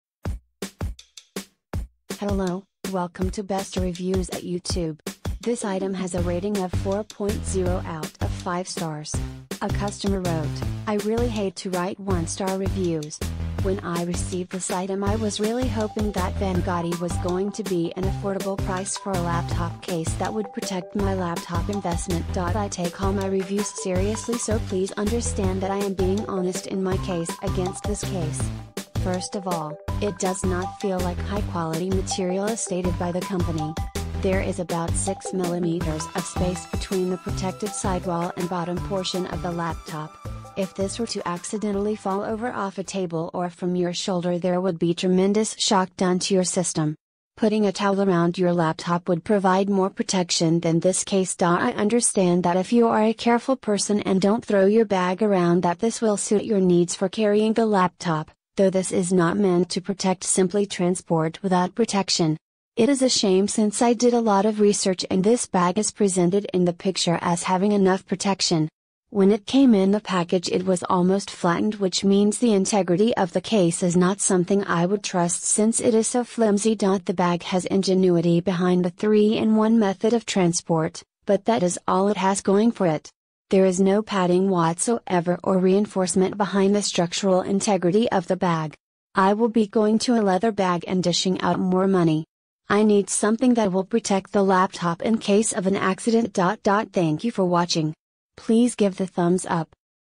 VanGoddy El Prado 3-in-1 Messenger + Backpack + Briefcase Transformer for 15 to 16 inch Laptops and
Review about   VanGoddy El Prado 3-in-1 Messenger + Backpack + Briefcase Transformer for 15 to 16 inch Laptops and Tablets - Black